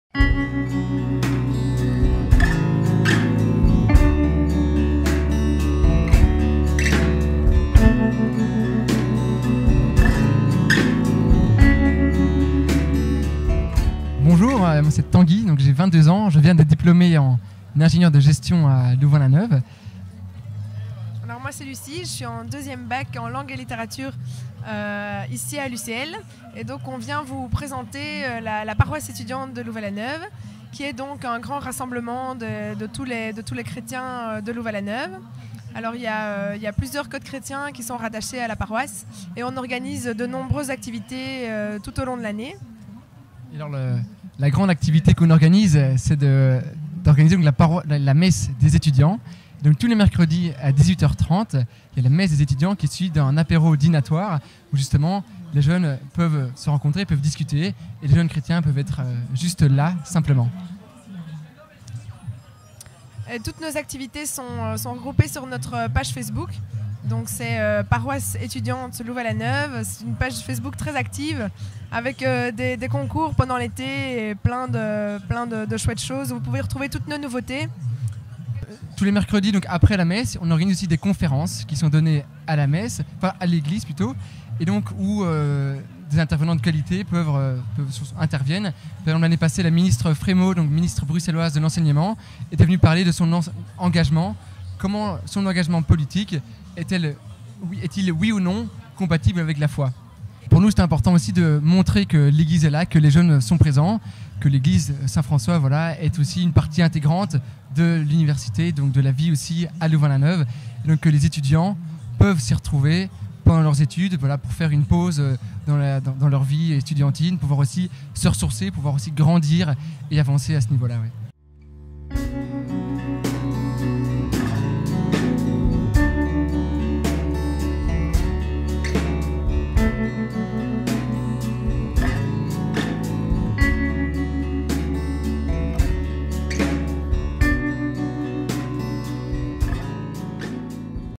Paroisse étudiante
La paroisse étudiante, c’est quelques centaines d’étudiants motivés par la fraternité et la joie de l’évangile.
Notre grand rassemblement est la messe des étudiants, chaque mercredi à 18h30 dans l’église Saint-François au bas de la rue de l’Hocaille. Ensuite un grand souper nous réunit dans le sous-sol de l’église. C’est une super-occasion de rencontrer des gens.